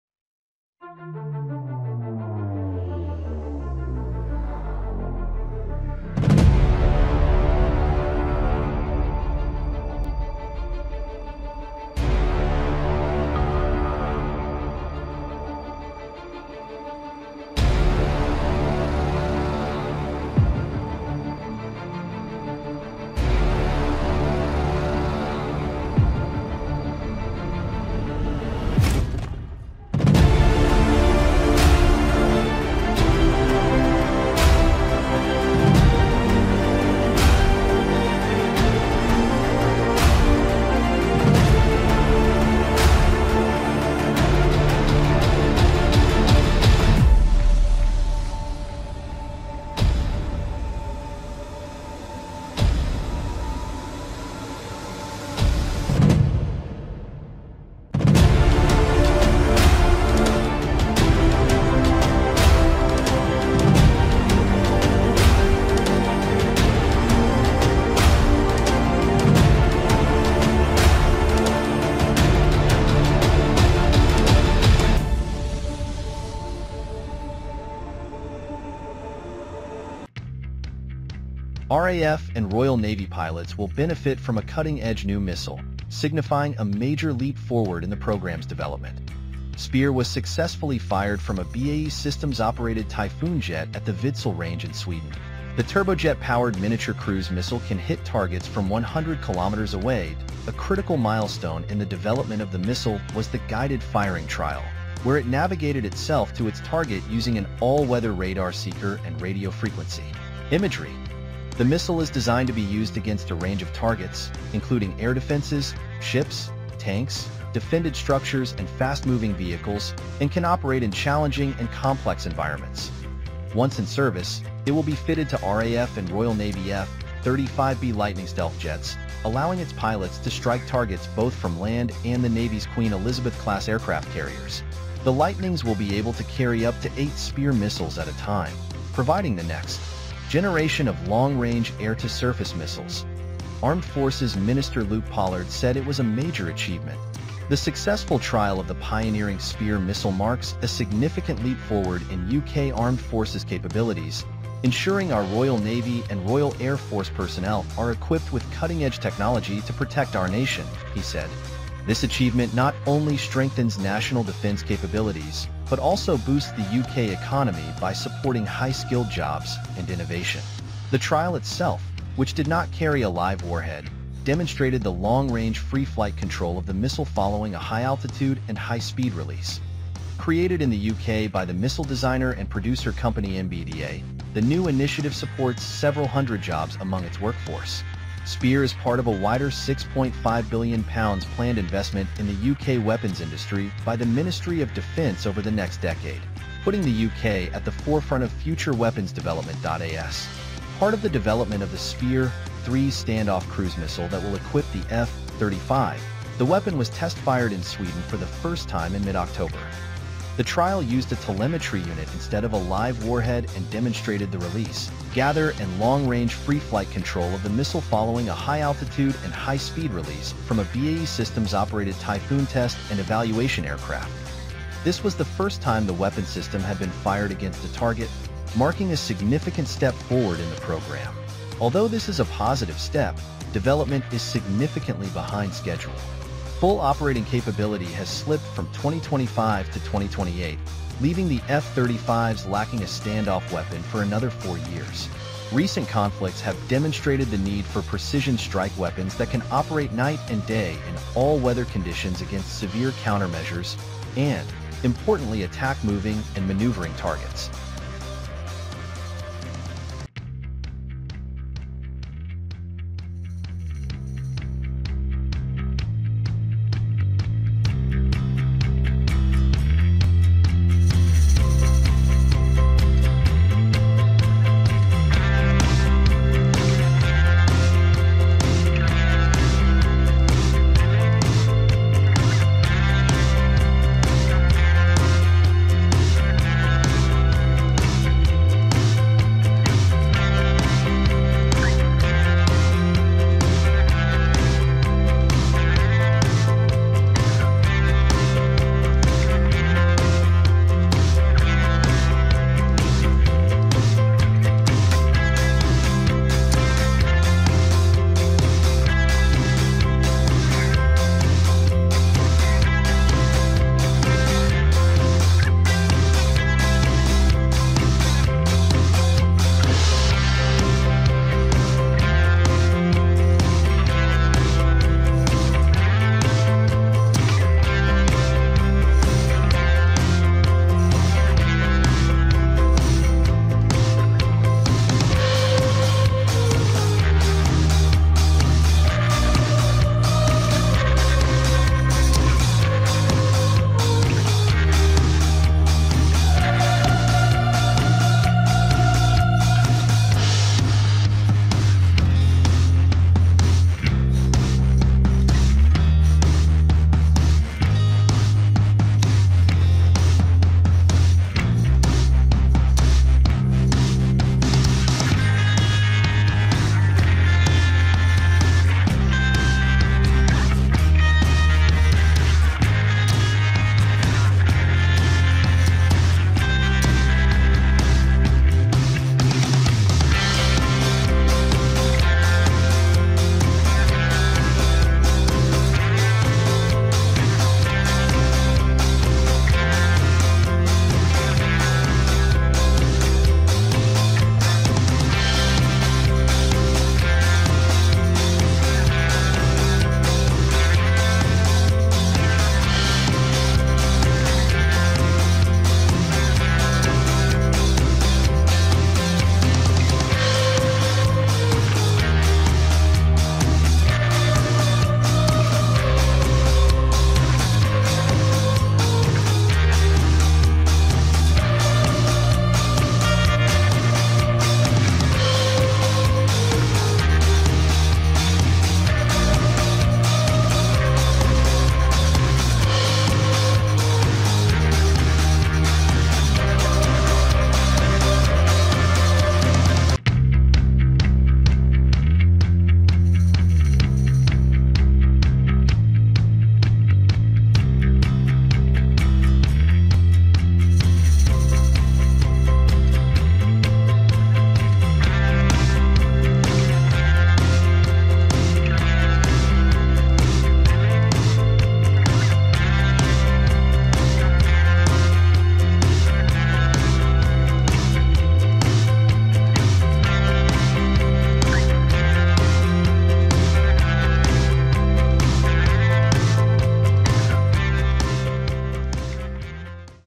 The Royal Air Force's first successful test of the F35 SPEAR-3 long-range missile
RAF and Royal Navy pilots will benefit from a cutting edge new missile, signifying a major leap forward in the programme's development.
Spear was successfully fired from a BAE Systems-operated Typhoon jet at the Vidsel range in Sweden.
The turbojet-powered miniature cruise missile can hit targets from 100km away